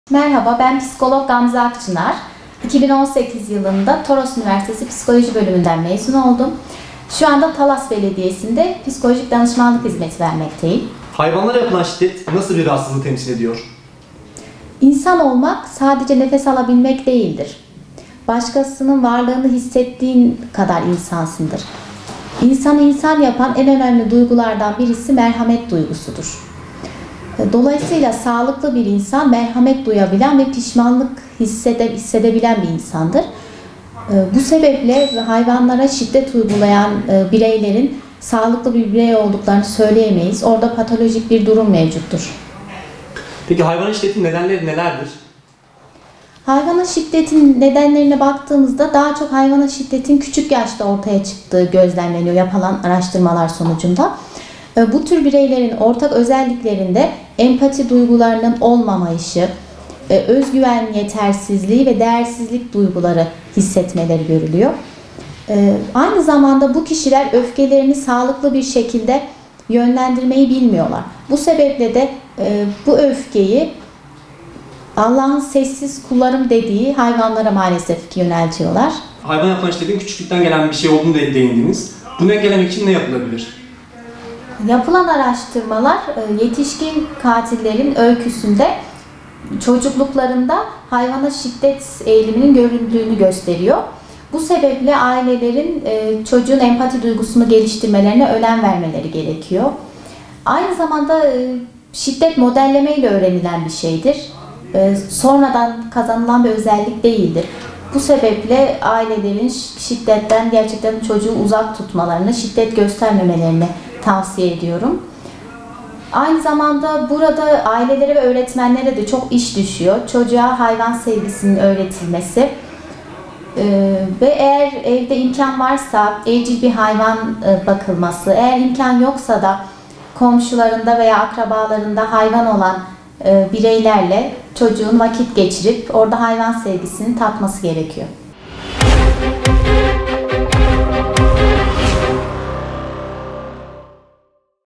Yaşam Onlarında Hakkı
Genel anket ve eğilimlere bakıldığında ya da doğrudan insanlara sorulduğunda, hepimiz hayvanları seviyoruz ve onları koruyoruz. Toplumun bireysel ifadelerine baktığımızda, hayvanlara uygulanan şiddet olsa olsa başka bir gezegenden geliyor diyebiliriz. Ama gerçeğimiz ne yazık ki aşikar. Son dönemlerde çokça duyduğumuz hayvanlara karşı şiddeti ve bununa altında yatan sebepleri, Psikolog Gamze Akçınar’a sorduk.
ÖNCE MERHAMET

Talas Belediyesinde görev yapan Psikolog Gamze Akçınar, bu tür davranışlar sağlıklı bireylerin sergileyeceği davranışlar olmadığına değinerek, ‘İnsanı insan yapan en önemli duygulardan birisi merhamet duygusudur. Dolayısıyla sağlıklı bir insan merhamet duyan ve pişmanlık hisseden insandır. Bu sebeple hayvanlara şiddet uygulayan bireylerin sağlıklı bir birey olduğunu söyleyemeyiz. O kişide Patolojik bir durum mevcuttur.’ dedi.
Hayvana Yapılan Şiddet Küçük Yaşta Ortaya Çıkıyor
Dünyada her gün sayısız hayvana şiddet uygulanıyor. Bu uygulanan şiddetlerin nedeniyle alakalı Psikolog Gamze Akçınar ‘Hayvana şiddetin nedenlerine baktığımızda, hayvana yapılan şiddetin yapılan araştırmalar sonucunda genellikle küçük yaşlarda ortaya çıktığı gözlemleniyor. Bu tür bireylerin ortak özellikleri empati duygularının olmayışı, özgüven yetersizliği ve değersizlik duyguları hissetmeleri. Aynı zamanda bu kişiler öfkelerini sağlıklı bir şekilde yönlendirmeyi bilmiyorlar.’ Diyerek insanların bu öfkeyi Allah’ın sessiz kullarım dediği hayvanlara yönettiklerini belirtti.
Hayvana Yapılan Şiddet Nasıl Engellenebilir?
Çocuklar şiddeti modellemeyle öğreniyor. Çocuğa şiddeti öğreten en birincil örnekler canlı modellerdir. Televizyon ve çizgi filmler ikinci plandadır. Bu nedenle şiddetin önüne geçmede çocuğun rol model olarak aldığı ebeveynlerinin davranışları, çocuklarına başka canlıların yaşam hakkına saygı duymasını öğretmesi en önemli etkendir. Hayvana Yapılan şiddetin engellenmesiyle ilgili Psikolog Gamze Akçınar ‘Yapılan araştırmalar yetişkin katillerin öyküsünde çocukluklarında hayvana şiddet eğiliminin görüldüğünü gösteriyor. Bu sebeple ailelerin çocuğun empati duygusunu geliştirmelerine önem göstermeleri gerekiyor. Aynı zamanda şiddet modelleme ile öğrenilen bir şeydir, sonradan kazanılan bir özellik değildir. Bu sebeple ailelerin çocuğu şiddetten uzak tutmalarını şiddet göstermemelerini tavsiye ediyorum. Burada ailelere ve öğretmenlere çok iş düşüyor. Çocuğa hayvan sevgisinin öğretilesi gerekir.’ Diyerek imkanları varsa evlerinde evcil bir hayvan bakılması gerektiği tavsiyesinde bulundu.